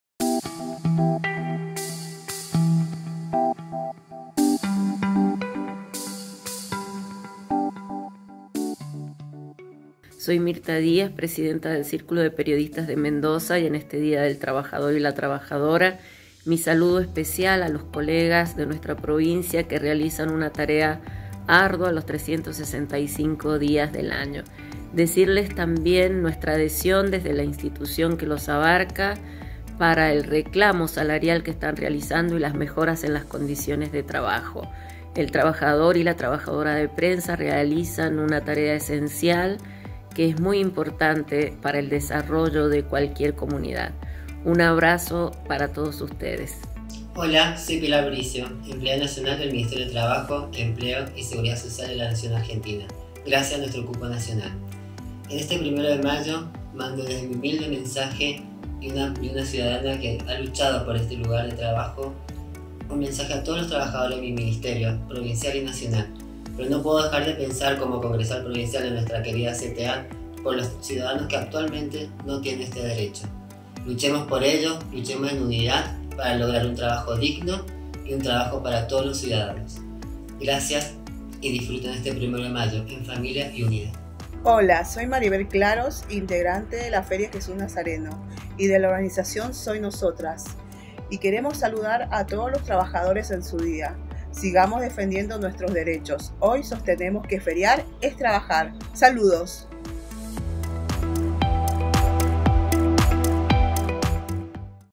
Saludos en el día de lxs trabajadores 2: Círculo de Periodistas, Min. de Trabajo y Feria Popular
En estos breves videos les compartimos saludos y reflexiones de trabajadores y trabajadoras organizadxs de Mendoza para este 1ro de mayo de 2023.